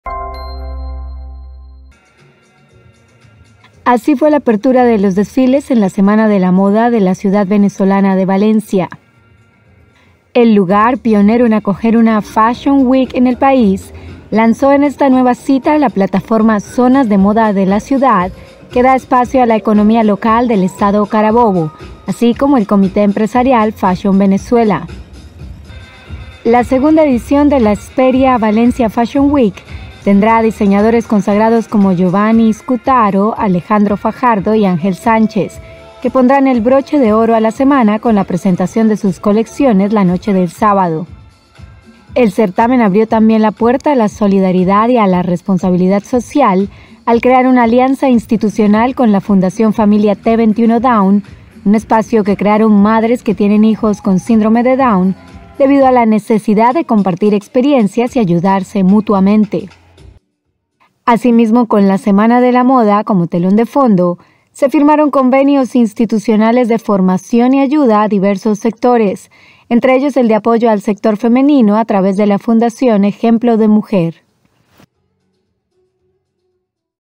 Venezuela busca "normalidad" con la semana de la moda en Valencia
Después de tres días de foros y conferencias, la Semana de la Moda de la ciudad venezolana de Valencia dio paso a los eventos estrella, con la apertura de los desfiles, cuya primera jornada, que comenzó la noche del jueves, concluyó pasada la medianoche de este viernes, horas antes de que expertos irrumpieran en el mismo espacio para hablar de tendencias en la estética facial.

En perfecta armonía con el mundo de la pasarela y bajo el título "La belleza está de moda", el cirujano plástico Rubén Pérez Parra y la dermatóloga María Alejandra Sarabia explicaron la importancia del cuidado de la piel y de "sentirse bien con uno mismo", en combinación con la elección de las prendas de vestir.